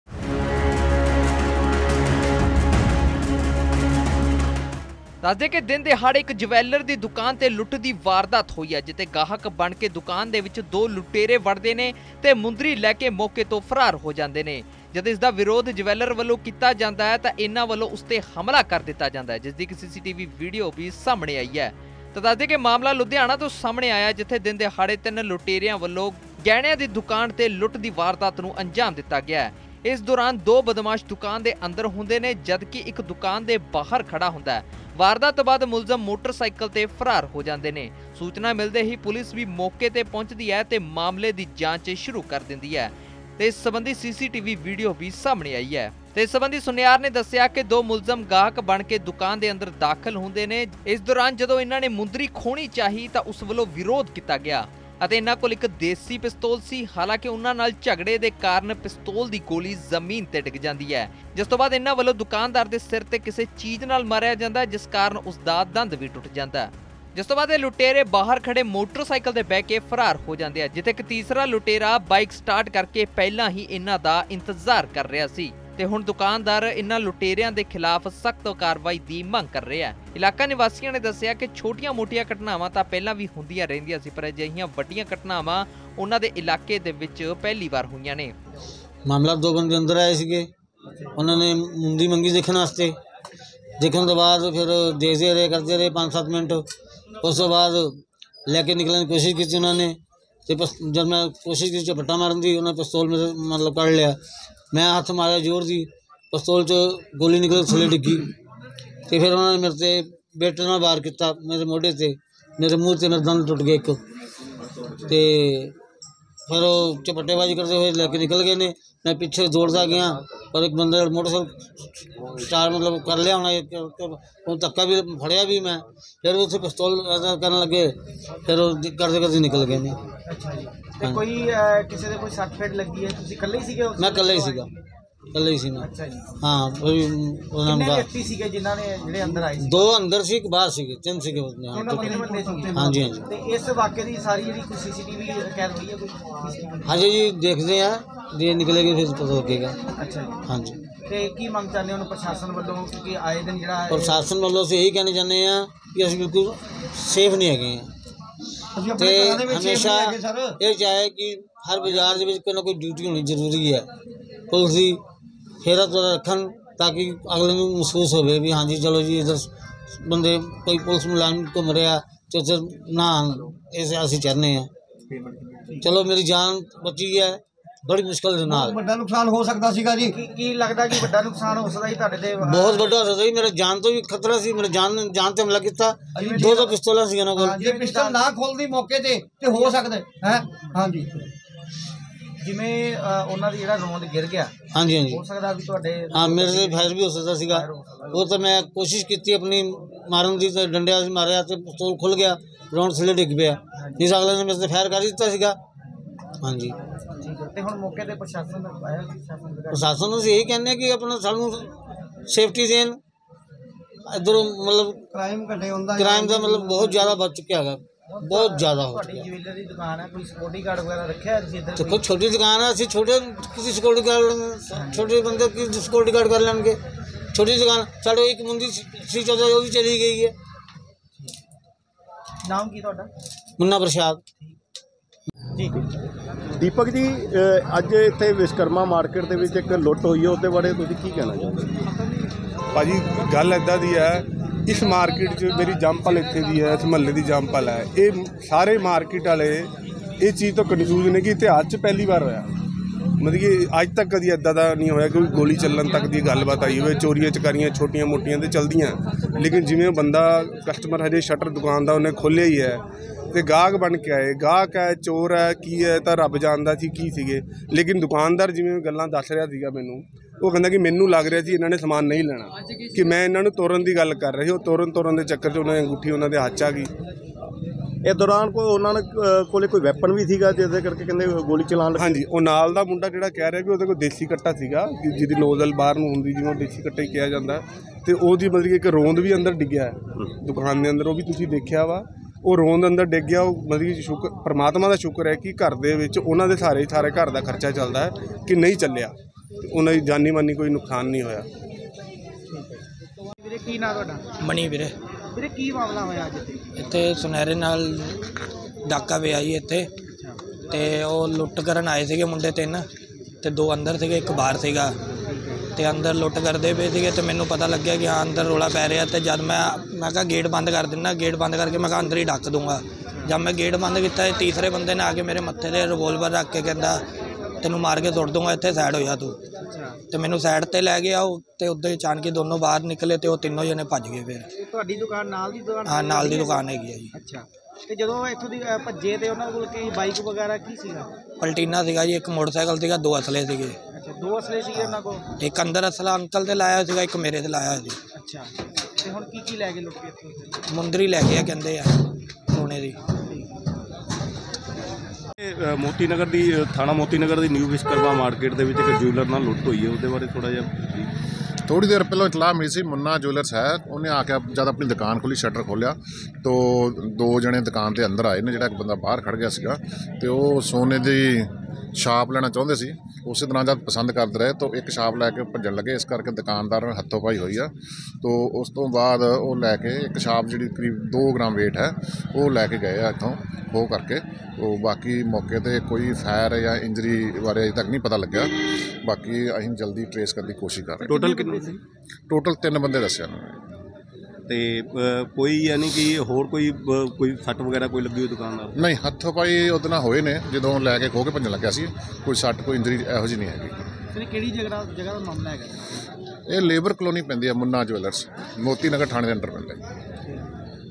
Loot In Ludhiana | ਸੁਨਿਆਰ ਦੀ ਦੁਕਾਨ ਤੋਂ ਮੁੰਦਰੀ ਖੋਹ ਕੇ ਭੱਜੇ ਲੁਟੇਰੇ
ਲੁਧਿਆਣਾ ਦੇ ਮੋਤੀ ਨਗਰ ਥਾਣਾ ਅਧੀਨ ਪੈਂਦੀ ਲੇਬਰ ਕਾਲੋਨੀ 'ਚ ਦਿਨ ਦਿਹਾੜੇ ਤਿੰਨ ਲੁਟੇਰਿਆਂ ਵੱਲੋਂ ਗਹਿਣਿਆਂ ਦੀ ਦੁਕਾਨ 'ਤੇ ਲੁੱਟ ਦੀ ਵਾਰਦਾਤ ਨੂੰ ਅੰਜਾਮ ਦਿੱਤਾ ਗਿਆ। ਇਸ ਦੌਰਾਨ ਦੋ ਬਦਮਾਸ਼ ਦੁਕਾਨ ਦੇ ਅੰਦਰ ਵੜੇ, ਜਦਕਿ ਇਕ ਦੁਕਾਨ ਦੇ ਬਾਹਰ ਹੀ ਰੁਕਿਆ ਰਿਹਾ।